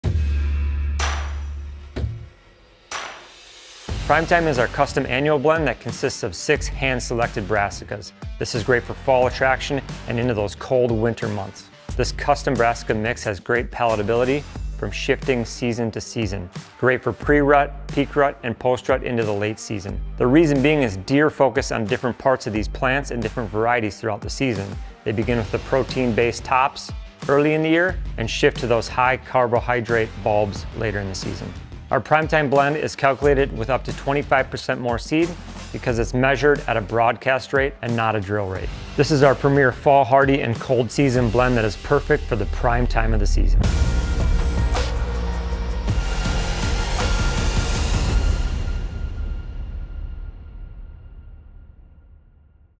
Prime Time Seed Blend - Illusion Food Plot Systems
Prime Time (Annual Seed Blend - 2lbs.): Prime time is built from six hand-selected varieties of premium brassica making it the ultimate and preferred food source during the Prime Time of hunting season.